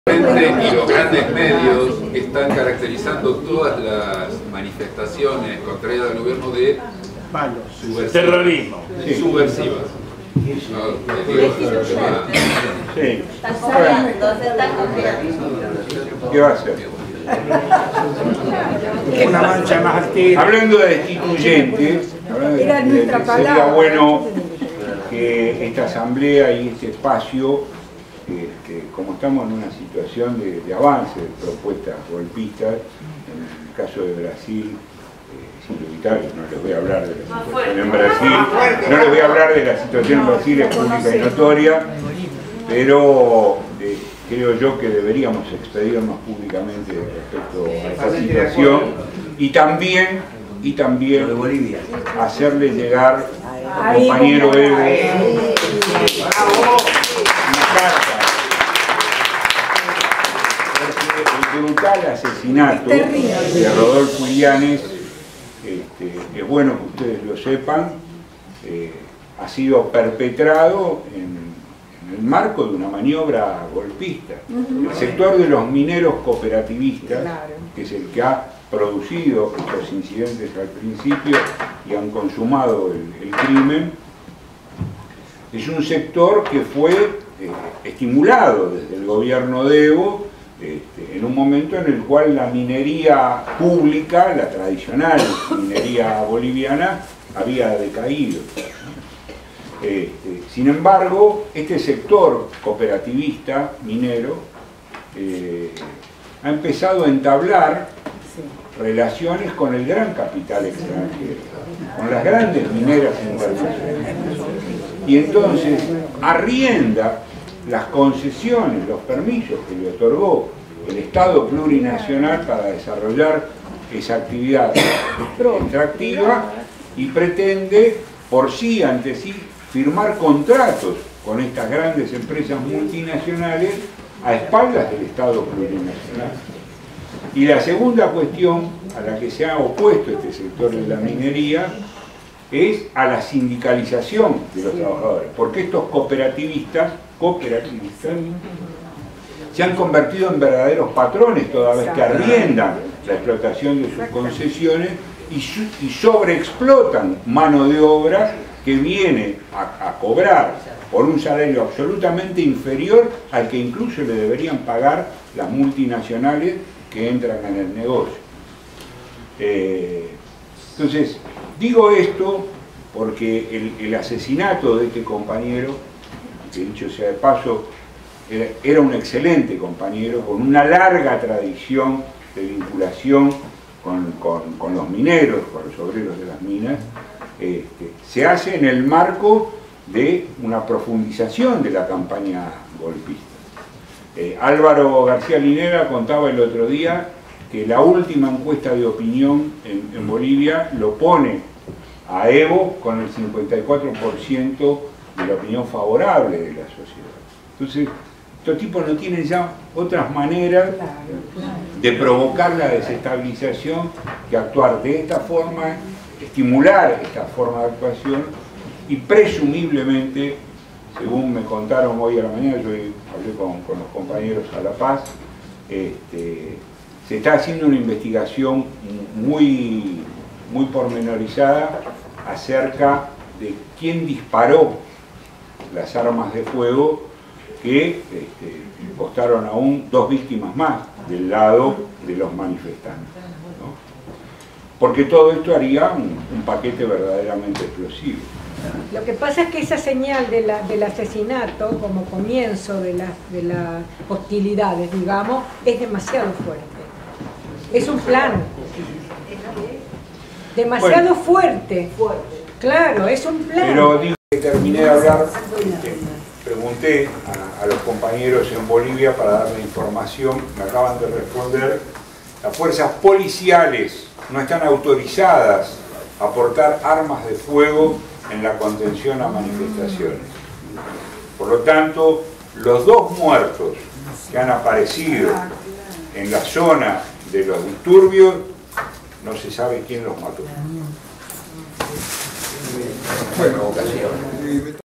05CA Carlos Girotti // desestabilización en Bolivia
Asamblea de Carta Abierta del sábado 27 de agosto de 2016. Facultad de Ciencias Sociales.UBA